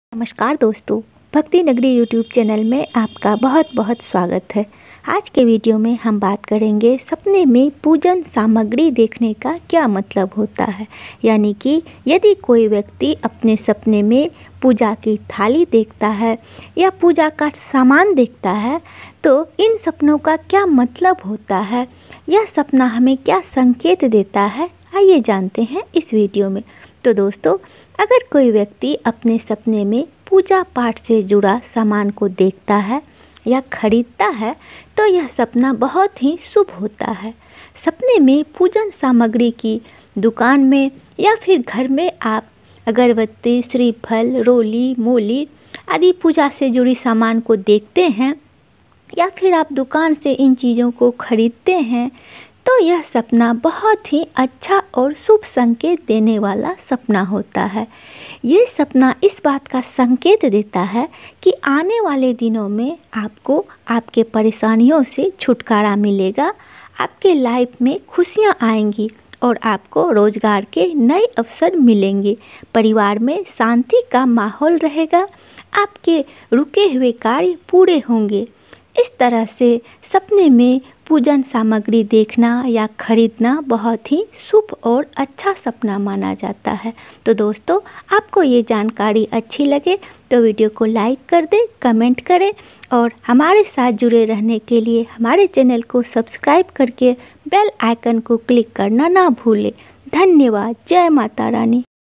सपने में पूजन सामग्री देखना /sapne me puja ka saman dekhna. Sapne me puja ki thali dekhna.
सपने में पूजा का सामान देखना/ पूजन सामग्री देखना/ पूजा की थाली देखना/इसके साथ हीं पूजा का सामान खरीदना इन सपनों का क्या मतलब होता है l
  आइये जानते हैं इस विडिओ में l

sapne me puja path ka sman dekhna
sapne me pujan samgri dekhna
sapne me pujan samagri kharidna
sapne me puja ki thali dekhna